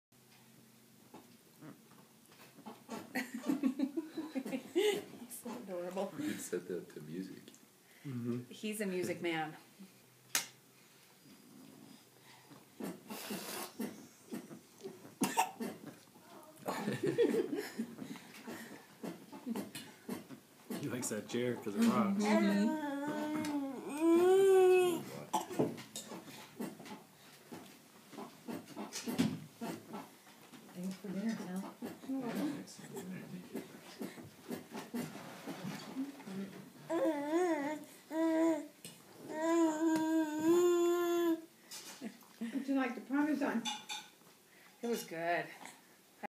Vyke Rocks the Chair
Grandson Vyke enjoys rocking the chair at lunch at Grandma Sally's.